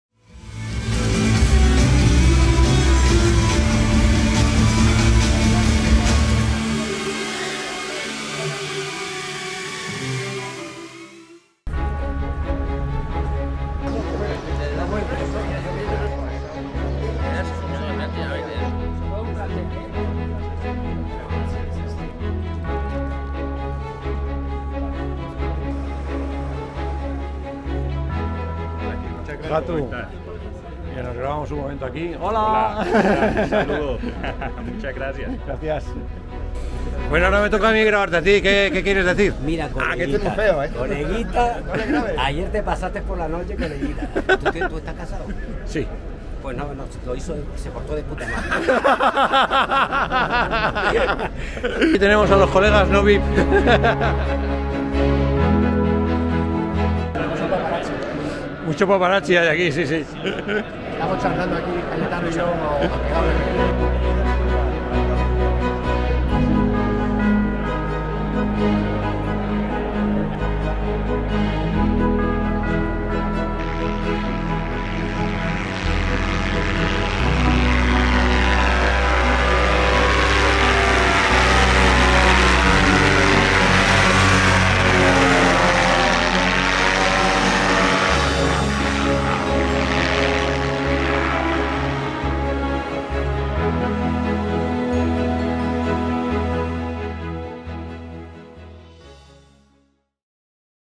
Future Drone 2022 Capítulo 1 (Lo más Importante)
Sin lugar a dudas, lo más importante del Future Drone 2022 han sido "Las Personas"... Convivencia, amabilidad, simpatía, colaboración... en resumen, gente "Genial".
Este es el primer capítulo de dos o tres que iré subiendo más adelante con alguna toma buena que haya salido de todo el material que he grabado.